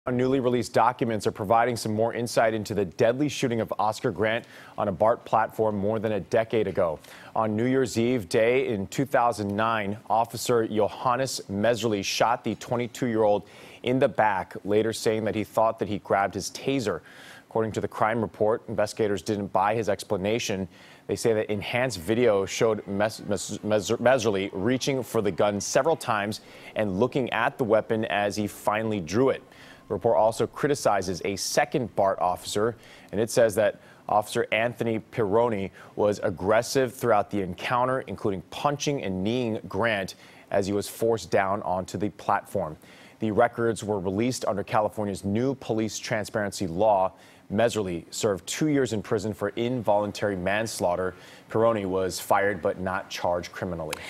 BART Releases Report On Oscar Grant Shooting That Sheds Light On Investigation
Newly released documents are providing more insight into the deadly shooting of Oscar Grant on a BART platform more than a decade ago.